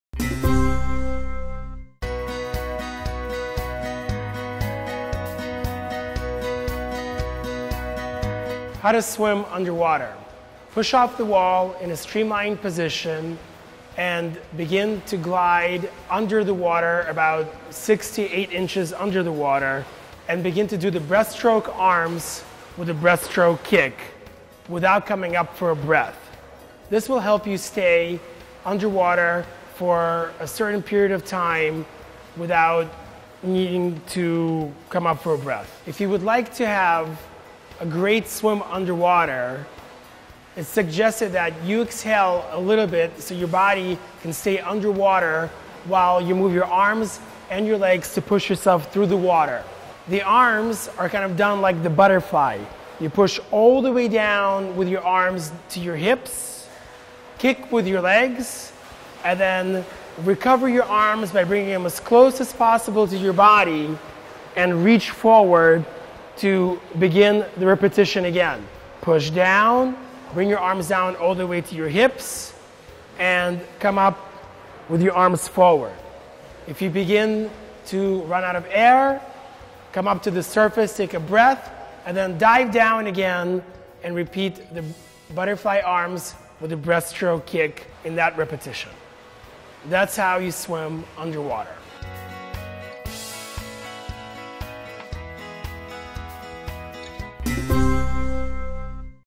How to Swim Underwater | Swimming Lessons
Amazon Must Haves for Every Swimmers:
Speedo Silicone Solid Swim Cap
TYR Socket Rockets 2.0 Racing Goggle
HEAD Energy Swim Fin
Speedo Deluxe Ventilator Mesh Equipment Bag

Just learning how to swim? Learn how to swim underwater with this online swimming lesson.

How to swim underwater.  Push off the wall in a streamlined position, and begin to glide under the water about 6 to 8 inches under the water, and begin to do the breaststroke arms with the breaststroke kick, without coming up for a breath. This will help you stay underwater for a certain period of time without needing to come up for a breath. If you would like to have a great swim underwater, it's suggested that you exhale a little bit so your body can stay underwater while you move your arms and your legs to push yourself through the water. The arms are kind of down like the butterfly.

You push all the way down with your arms to your hips, kick with your legs, and then recover your arms by bringing them as close as possible to your body, and reach forward to begin the repetition again. Push down, bring your arms down all the way to your hips, and come up with your arms forward. If you begin to run out of air, come up to the surface, take a breath, and then dive down again and repeat the butterfly arms with the breaststroke kick in that repetition. That's how you swim underwater.